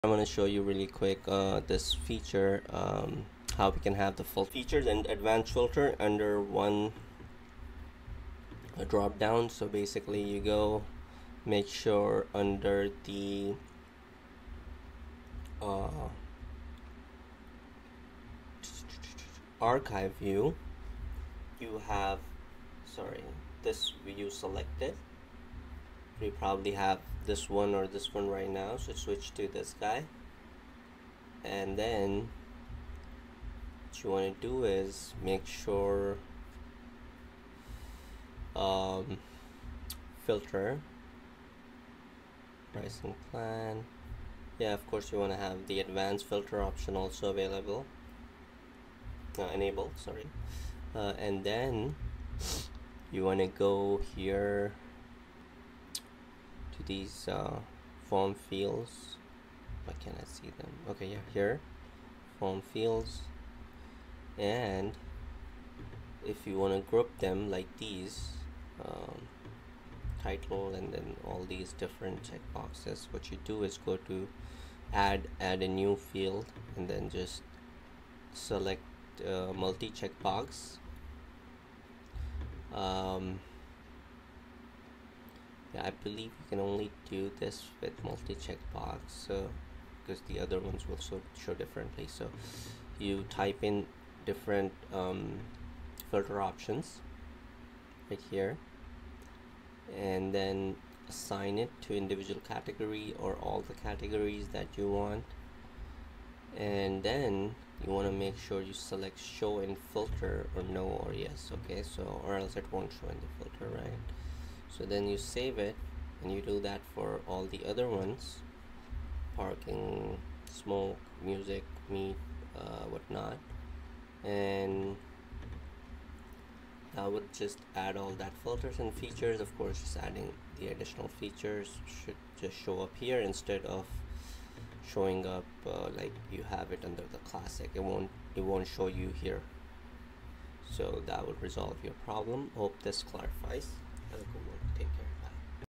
How to have Listing Features and Custom Form Fields show in Filter
In this video we will show you how to show both the Listing Features (Amenities) and Custom Form Fields (Additional Details) show in the search Filter inside a compact Additional Filter options box.

Interested to purchase ListingProWP?